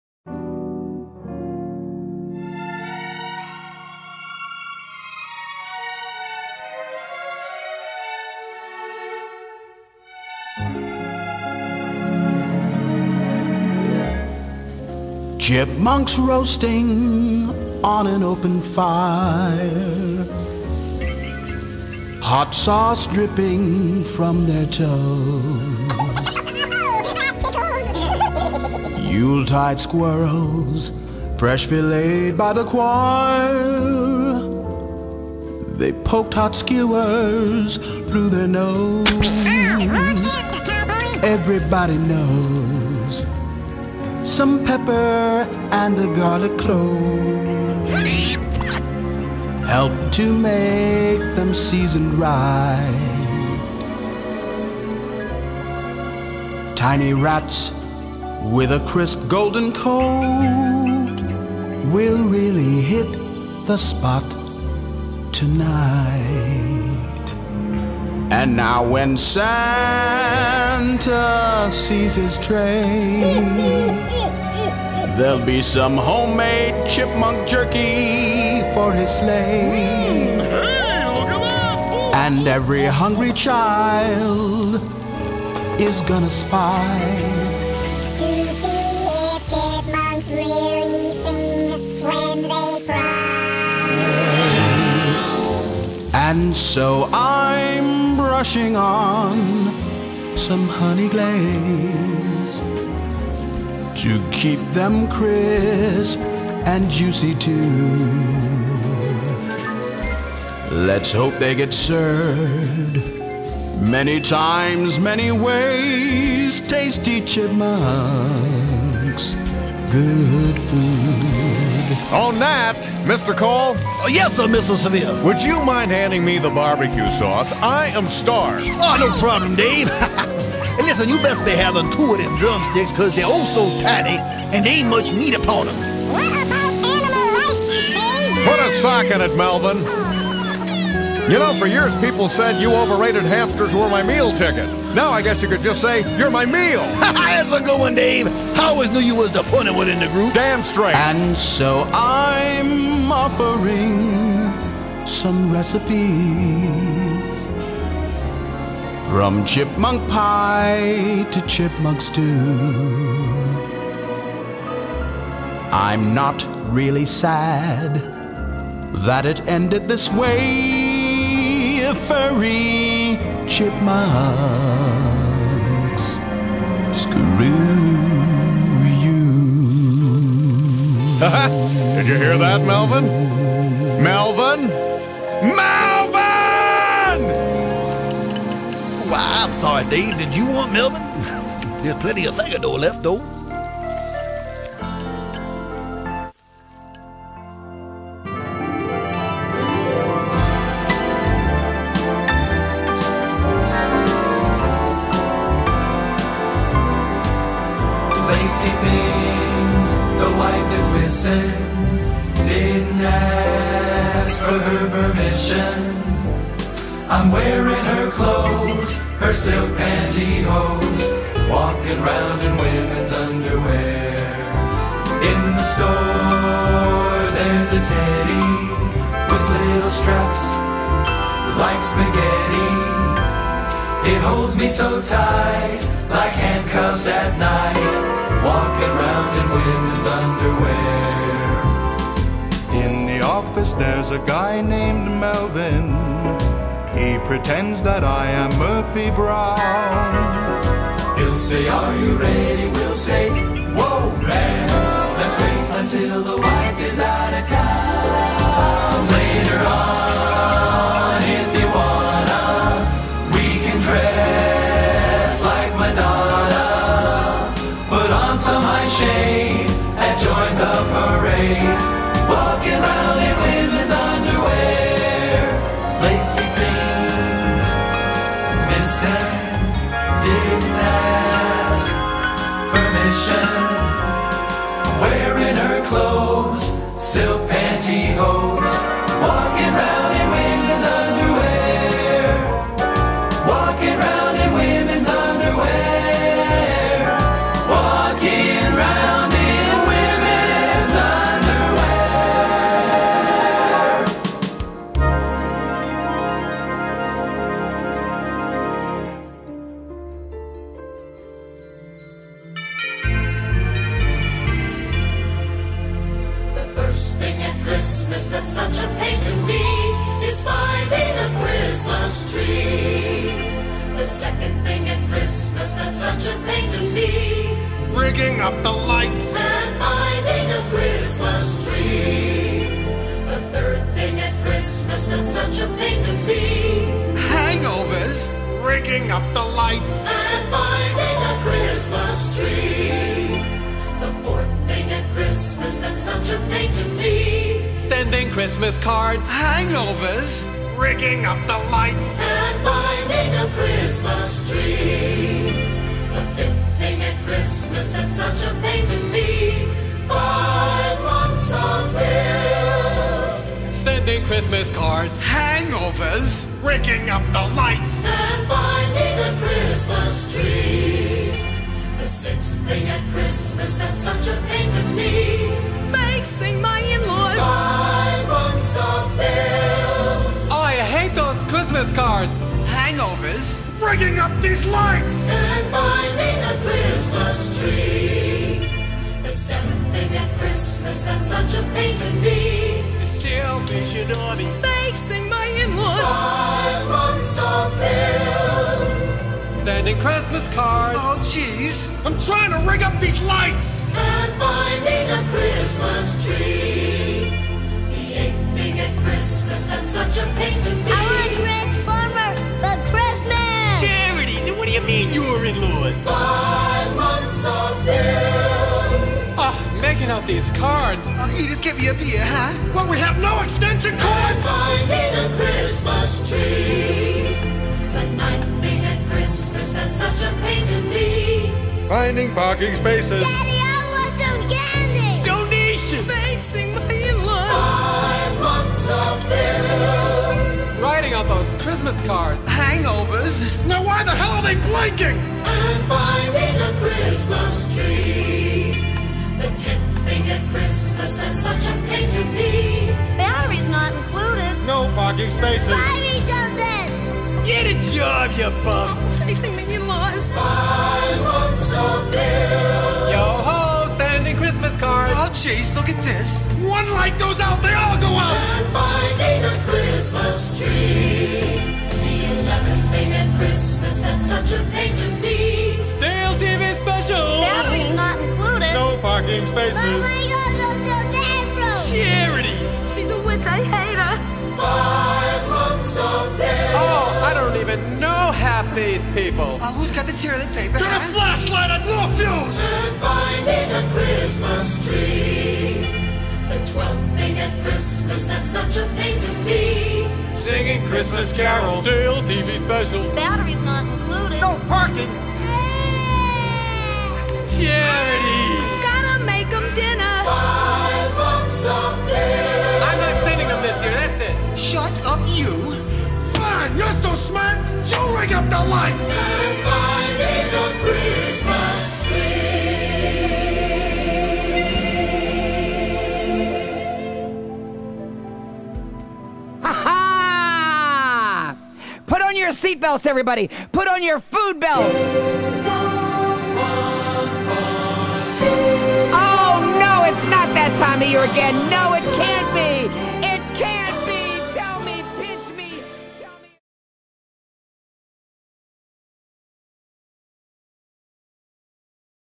Special Edit: Curacao Diving 2014 (Christmas Themed)
This video was created from underwater reef clips from our 2014 dive trip to Curacao.  The videos were shot with a GOPRO 3+.  This was also put togeter with a Christmas theme as a special edit.  Music is Christmas parody songs.